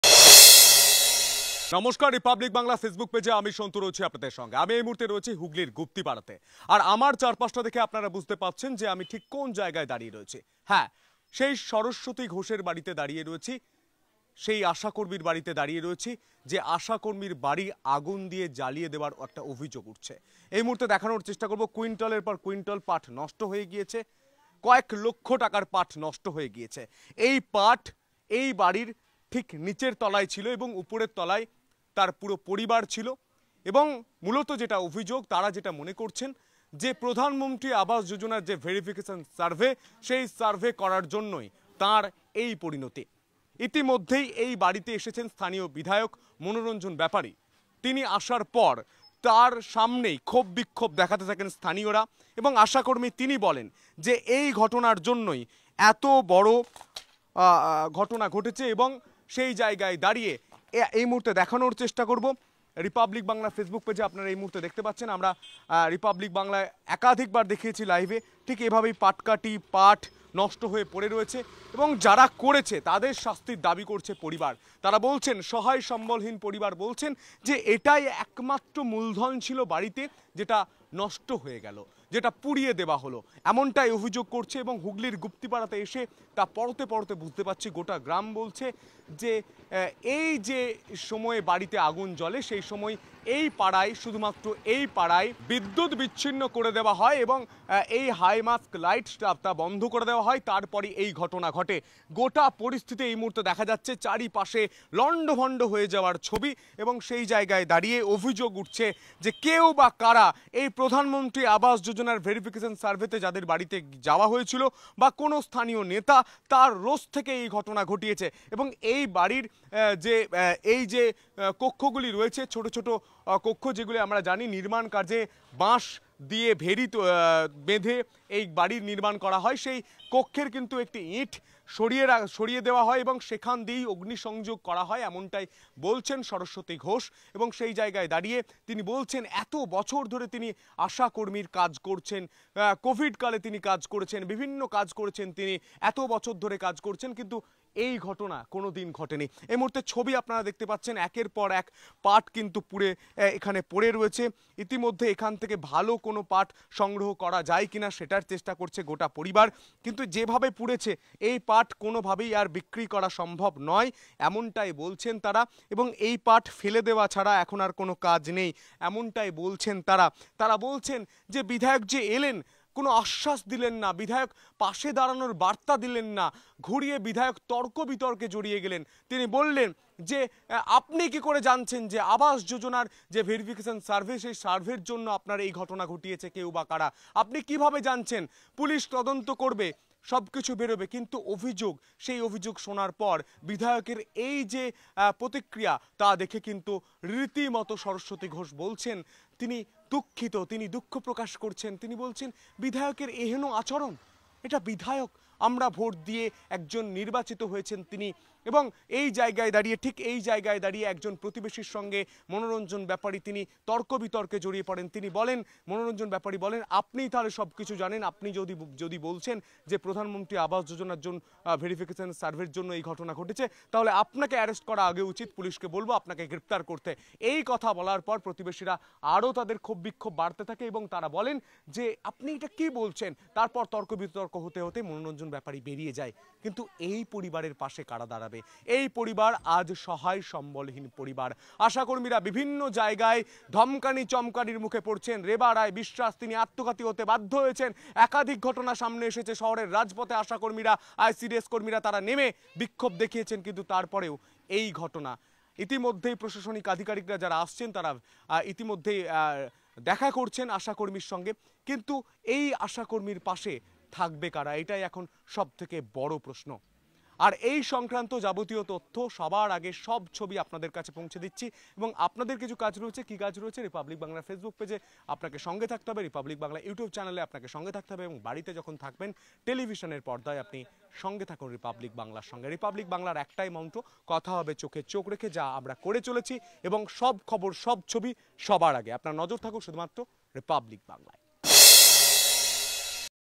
Asha Karmi | অঙ্গনওয়াড়ি কর্মীর দেহ উদ্ধারের পর এবার আশাকর্মী সরস্বতী ঘোষের পাটের গুদামে আগুন!
আজ নজরে: স্বরূপনগরের পর বলাগড়। অঙ্গনওয়াড়ি কর্মীর দেহ উদ্ধারের পর এবার আশাকর্মী সরস্বতী ঘোষের পাটের গুদামে আগুন! আবাস যোজনার সার্ভের কারণেই হামলা বলে অভিযোগ আশাকর্মীর

রিপাবলিক স্বাধীন।
রিপাবলিক নির্ভীক।
আপনিই রিপাবলিক।